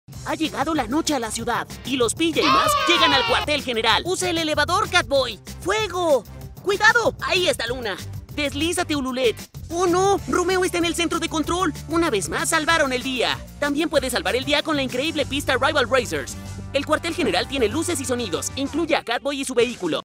PJ Masks - PJ Masks Cuartel General y Pista Rival Racers!
Ninja Nocturno ha entrado al museo en la pista Rival Racers, ¿podrán los PJ Masks detenerlo a tiempo? ¡Salva el día con la nueva pista Rival Racers!

Cae la noche en la ciudad y los PJ Masks luchan contra los villanos en el Cuartel General. Atrapa a Romeo, Luna y Ninja Nocturno en el Centro de Control usando el elevador, derribalos con la pelota y deslízate por la rampa. ¡Es hora de ser un héroe!                                           El Cuartel General incluye luces y sonidos, y una figura de Catboy y su gatomóvil.